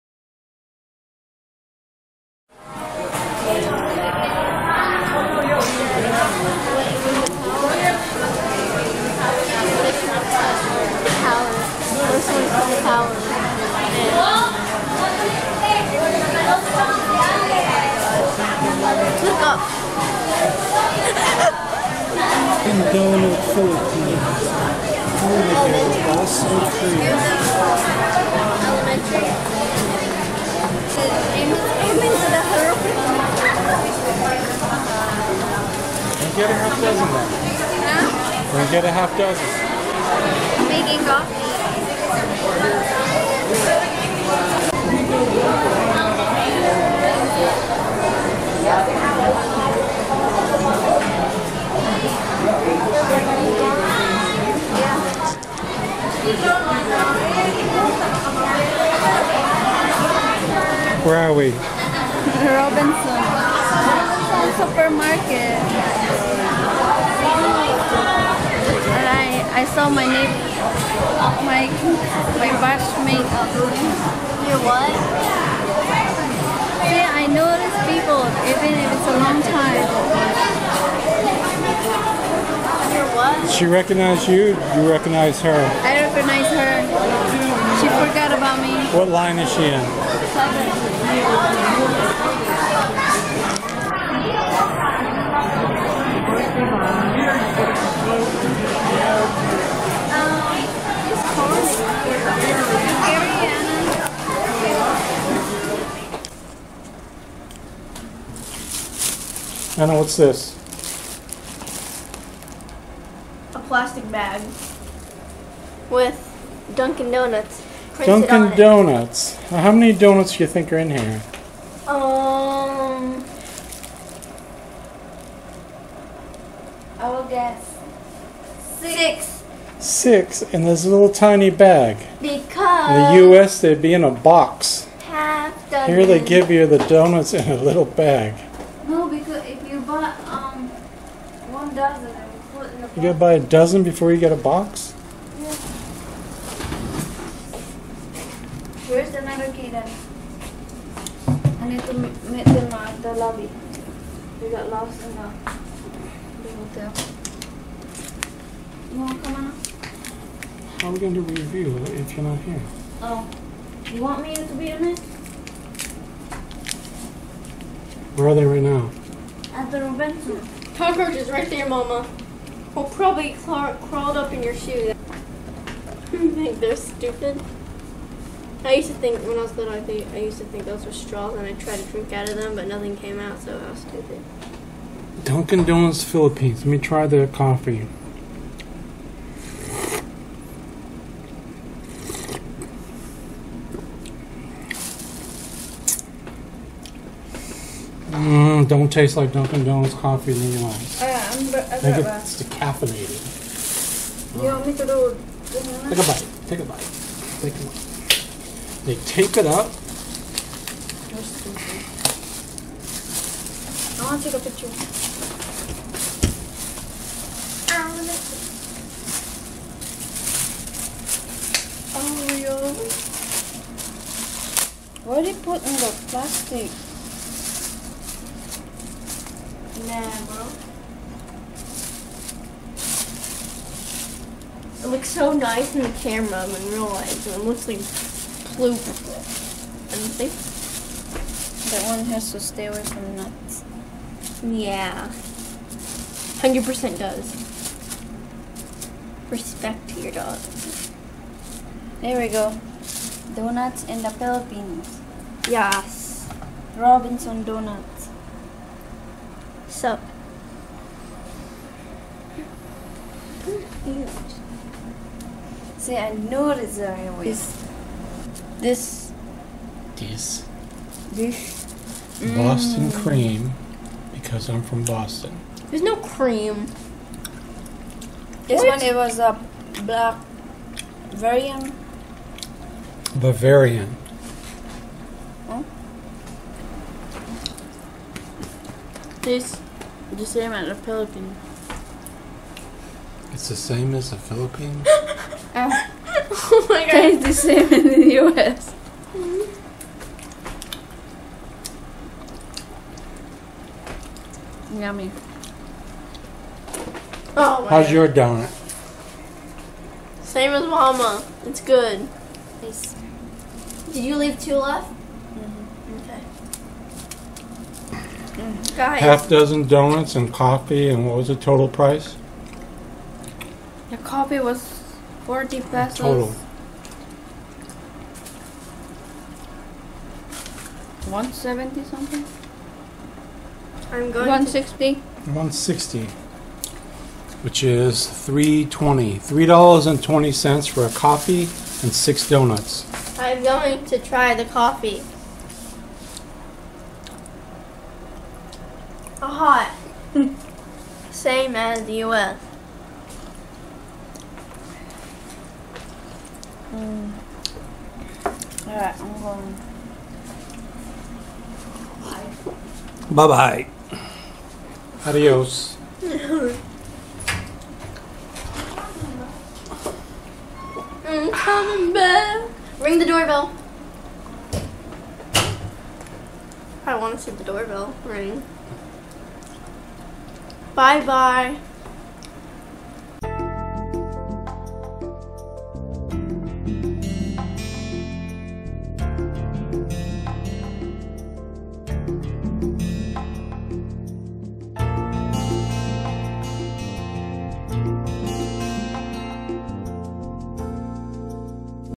Dunkin Donuts Philippines
Dunkin Donuts Philippines. We stopped by Dunkin Donuts while we were in Robinson's Mall in Cebu Philippines.